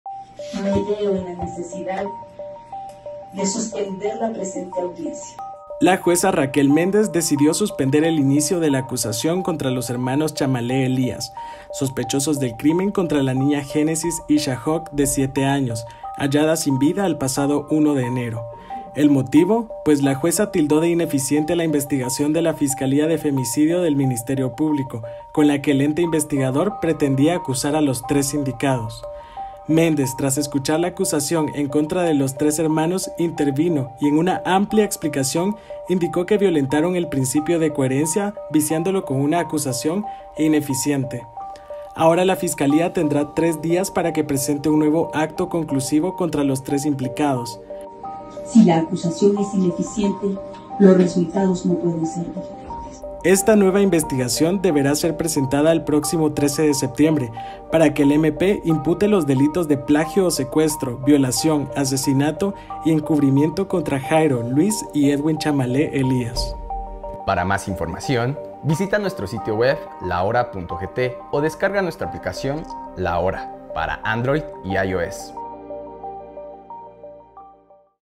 Caso Génesis: Jueza señala «investigación ineficiente» del MP contra sospechosos
La jueza Raquel Méndez decidió suspender el inicio de la etapa intermedia contra los hermanos Chamalé. Te contamos los detalles 📹